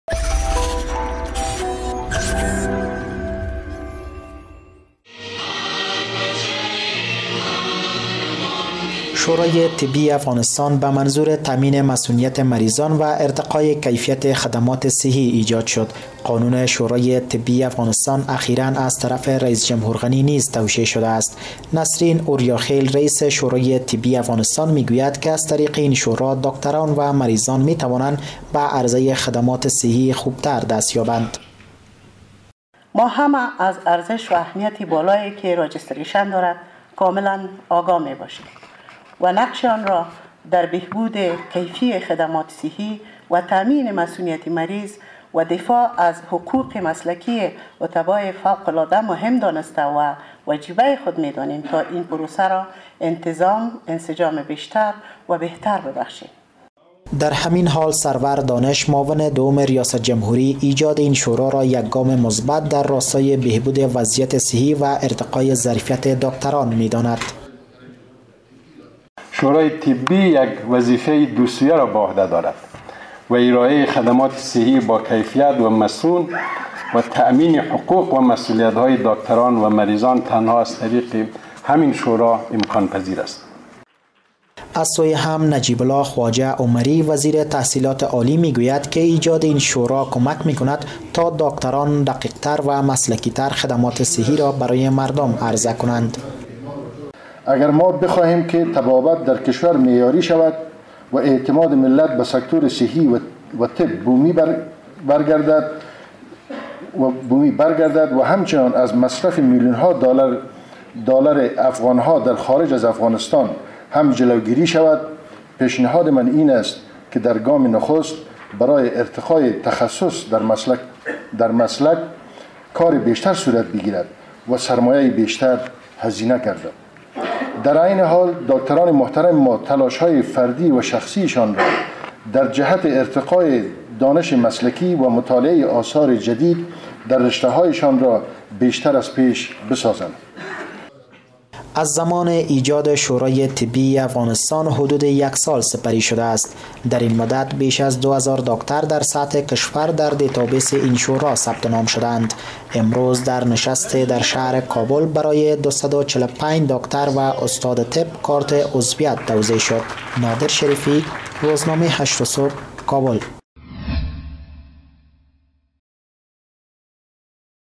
امروز در نشستی در شهر کابل برای 245 داکتر و استاد طب، کارت عضویت توزیع شد
شواری طبی افغانستان به منظور بلند بردن میزان رضایت مردم نسبت به نظام صحی کشور
امروز در نشستی در شهر کابل برای 245 داکتر و استاد طب، کارت عضویت توزیع کرد